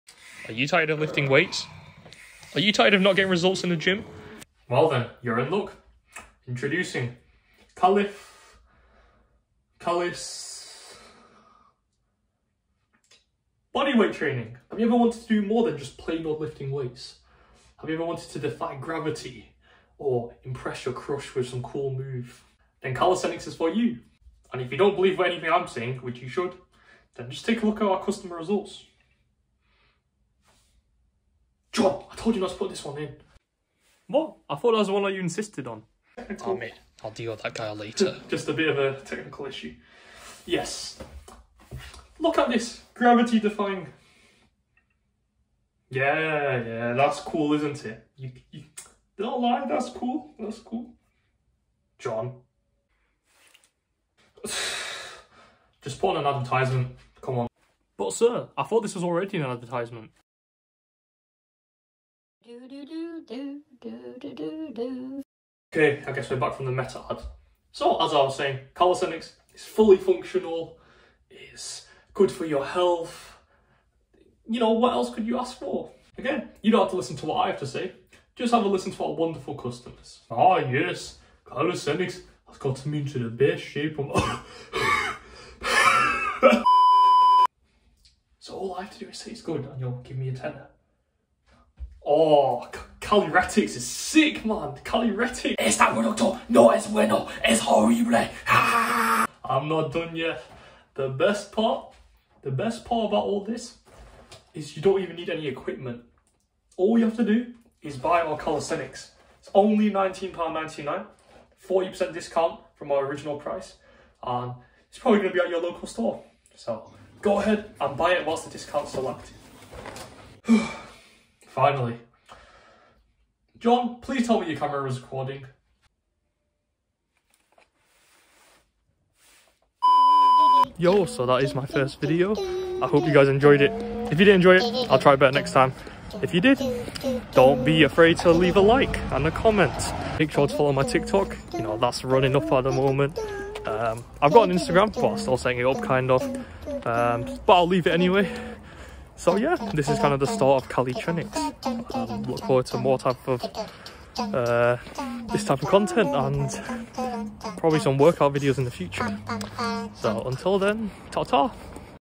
If calisthenics was an ad...
This was so much fun to make, though I'm probably going to look back in 2 days and cringe.

My name is Chenny , and I like calisthenics! Join me on my journey and grow together.